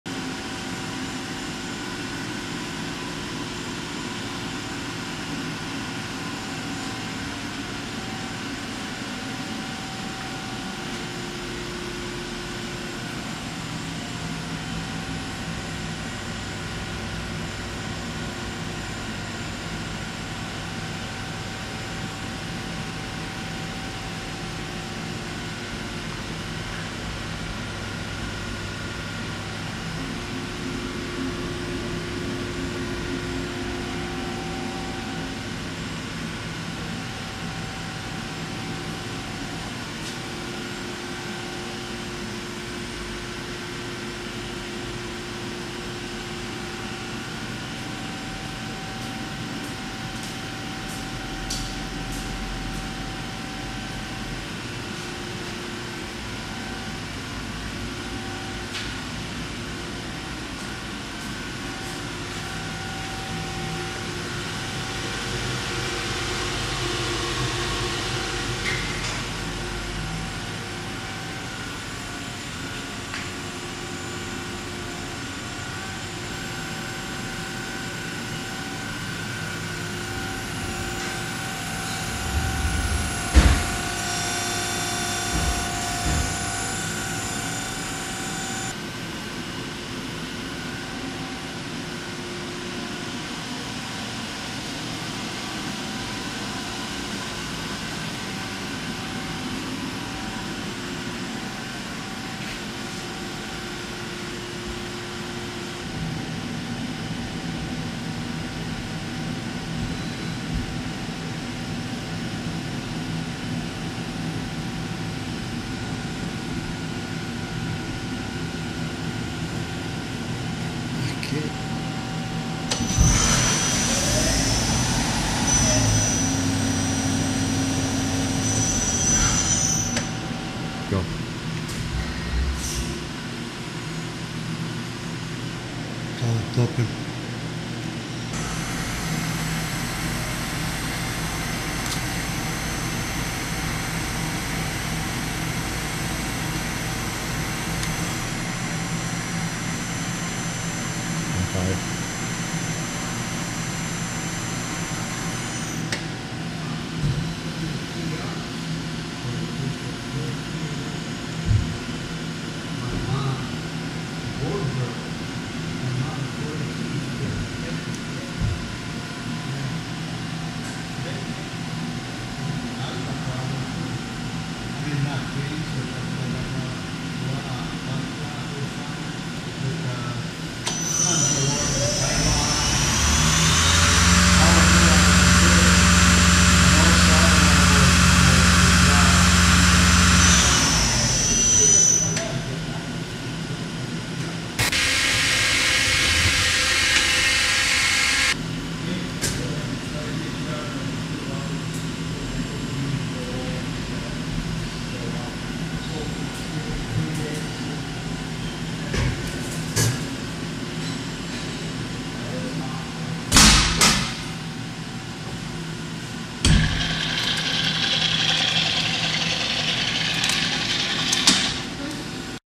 Eurotech 730-SLY CNC Lathe Turning Center- CNC DIgital, Inc.
Eurotech 730-SLY
Machine Specifications:
Bar Capacity:      2.625"
Max. Mach. Dia:      9.842"
Max. mach. length:     19.685"
Max. Swing:     20.472"
Main Spindle:
Max. Spindle RPM:     4000
Spindle Nose:     A2-6
Motor HP:     22/26 Kw
Sub Spindle:
Spindle Nose:     A2-5
I.D. drawtube:    1.692"
Motor HP:      5.5/7.5 Kw
Turret:
No. of Stations     12
Square shank tooling:     3/4"
I.D. Tool holders:    1" & 1.25"
Index time 1 Pos.:     0.4 sec.
Live Tooling:
Live Tool Pos.     12
Max. RPM:      3000
Motor HP:      3.7 Kw
C- Axis on Main Spindle:
Type:     CS (direct)
Min. Prog. Value:     .001 deg
Rapid Trav.RPM:     100
Axes Rapid Traverse IPM:
(X)      472
(Z)      787
(B)      944
(Y)      236
Cooling System:
Tank Cap. in Gallons:     52
Pump Displacement Gal./Min.    26
Machine Dimensions:with chip conveyor: L158" x W78" x H85"
Weight Lbs.:     11684